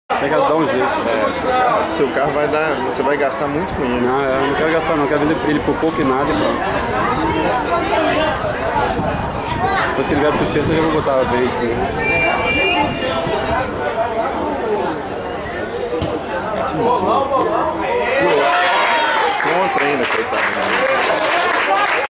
Aristides
gol contra , falta cobrada pelo Ari